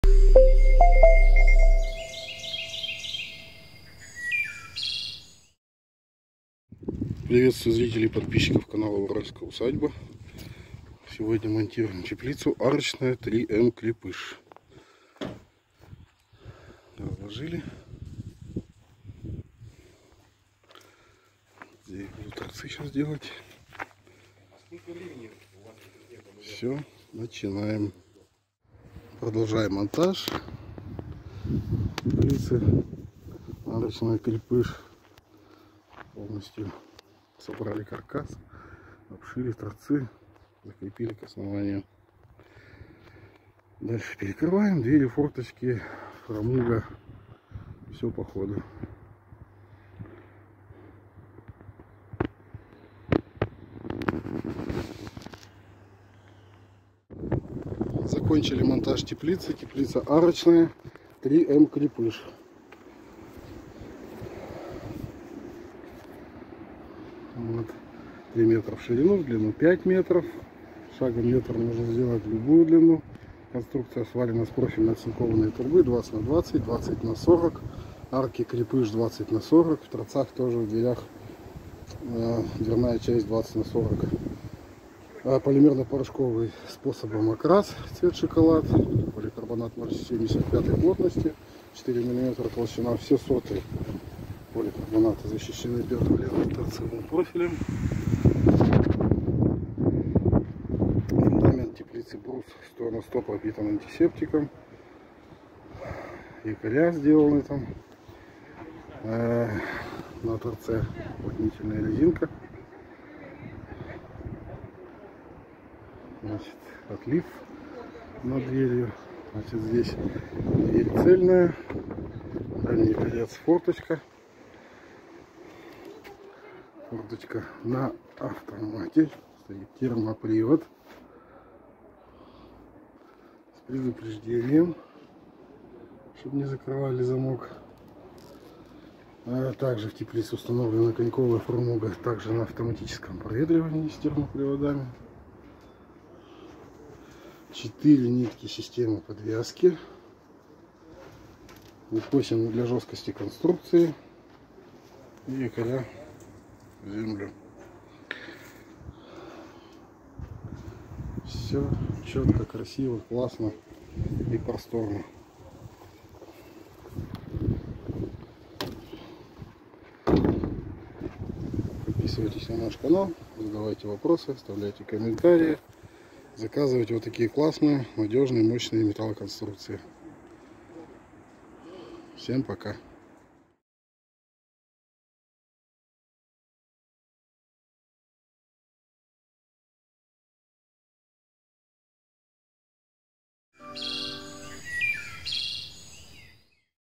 ТЕПЛИЦА С ГАРАНТИЕЙ СНЕГ НЕ ЧИСТИТЬ АРОЧНАЯ 3М КРЕПЫШ
АРОЧНАЯ 3М КРЕПЫШ ТЕПЛИЦА.

Сейчас в этом видео, вы увидите и удивитесь обычная арочная теплица из трубы 20х20х1,1 и 20х40х1,4мм  и выдерживает любые снеговые нагрузки без ограничений при шаге в 1 метр.
И при этом мы даём гарантию 10 лет👌

Как так вы спросите?
Все очень просто, сечение или толщина трубы 1,4мм.